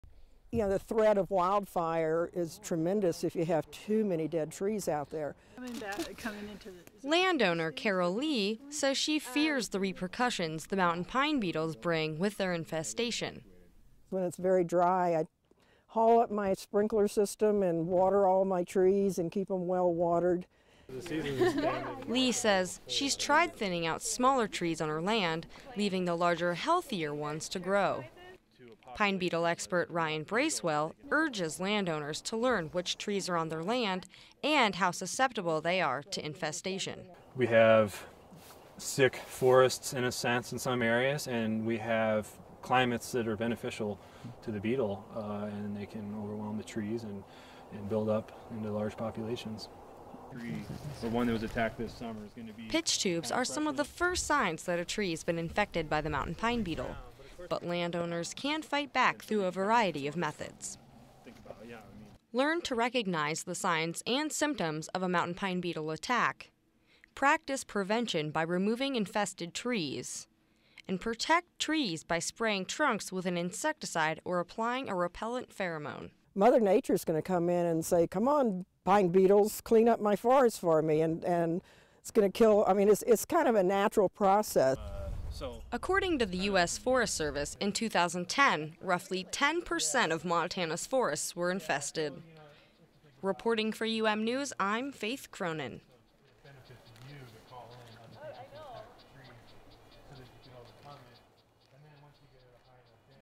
Montana landowners fight back against the Mountain Pine Beetle.
Montana land owner, Karole Lee, says it's important that land owners remember that they the stewards of the land. Lee owns five and a half acres in the Clancy area and says she strives to maintain the trees on her property; because healthier trees are less likely to be infested by mountain pine beetles. Lee advises other land owners to keep their trees well watered, as well as to remove the smaller weaker trees to enable the larger healthier ones to grow. UM doctoral student and pine beetle expert, Ryan Bracewell, says it's important that land owners can identify the trees on their land and how susceptible they are to pine beetle infestation. Reporter and photographer Faith Cronin and photographer Scott Ranf dug a little deeper to find out what methods land owners can rely on to get rid of pine beetle infestations.